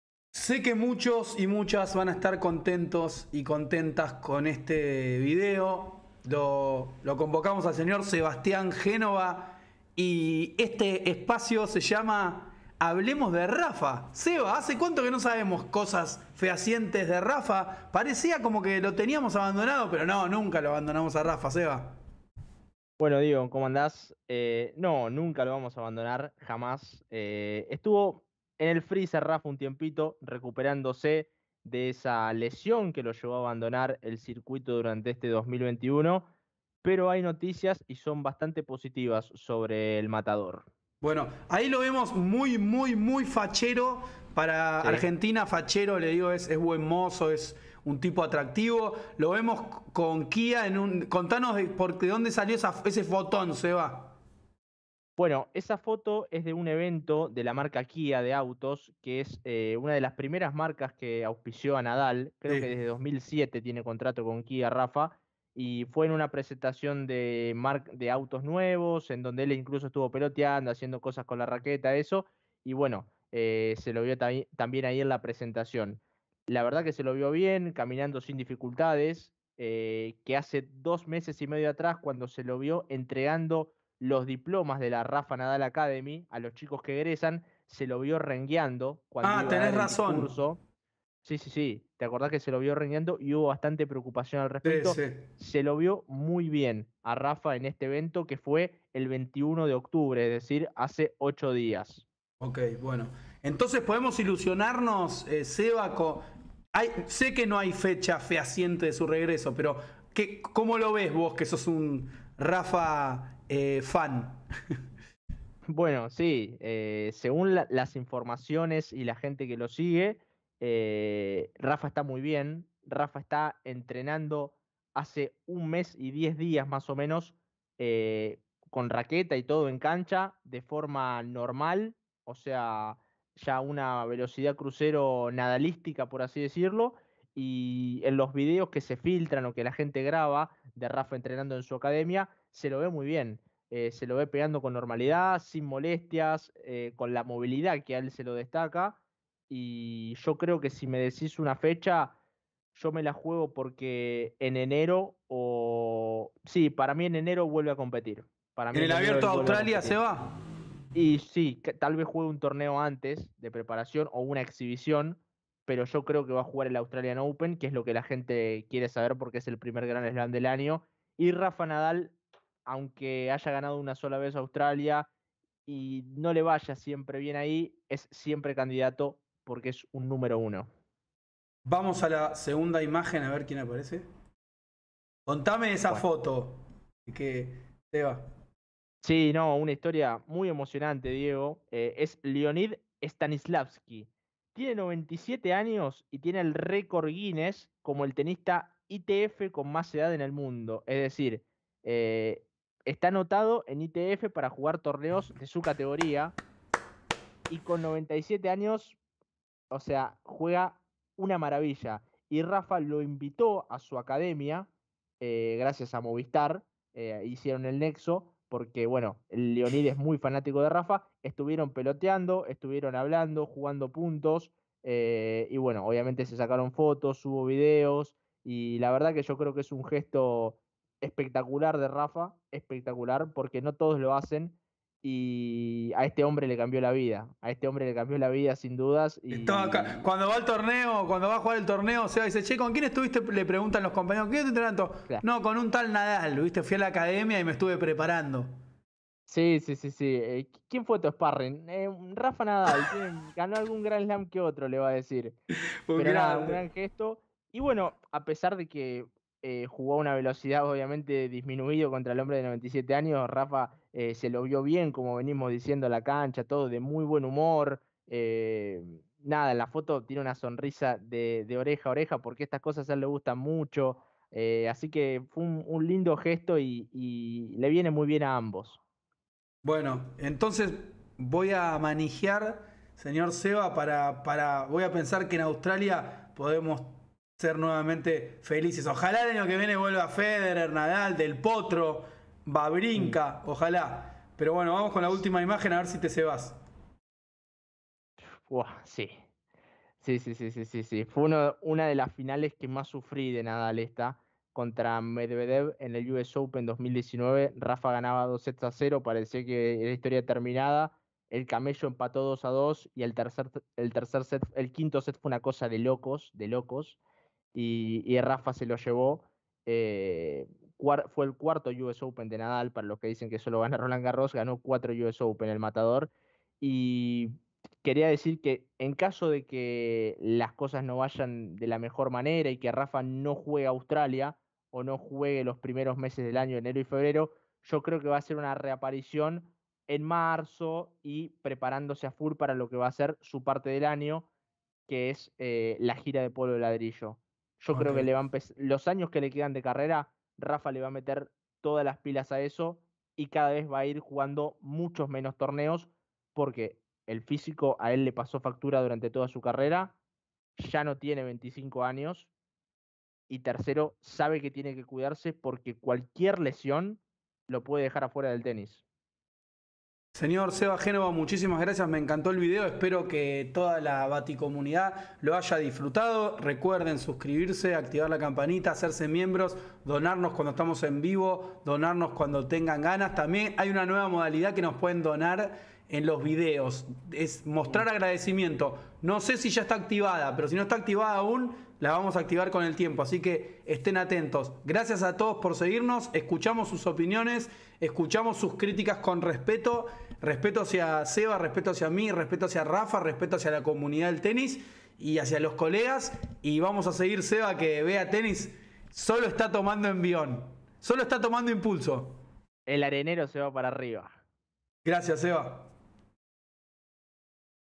¿Cuándo vuelve Rafael Nadal?
¿Qué sabemos del retorno del MATADOR Rafael Nadal?
La manija y energía de Sebastián Génova junto a Diego Amuy.

Si te gusta nuestro contenido y querés donarnos el equivalente a un cafecito, y con Mercado Pago...